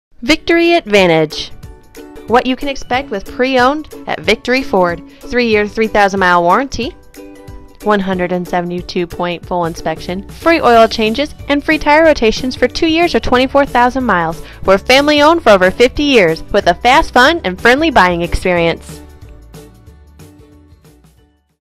Dyersville Advantage Maintenance Program | Victory Ford IA
Come on in to visit us at Victory Ford to find out all about our Advantage Maintenance Program for your pre-owned vehicle which offers:
-3 year/ 3,000 mile warranty (for select pre-owned vehicles)
-172 point full inspection
-Free Oil changes & Free tire rotations for 2 years / 24,000 miles

For more information about this great offer, come on in to visit us at 30584 Olde Hawkeye Road in Dyersville, IA 52040 or give the service center a call at .